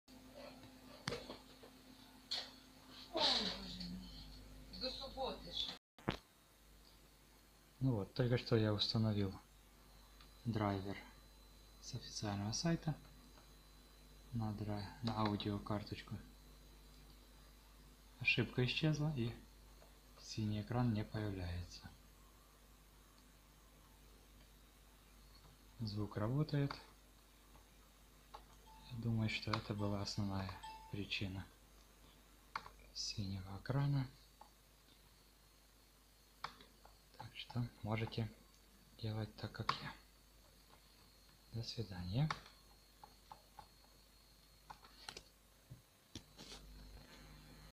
memory parity error - solutions to problem. Video part 5
solutions to problems set the driver Sound cards with the official website.Video Part 5 for fix this error memory parity error - download original driver for sound card and install it...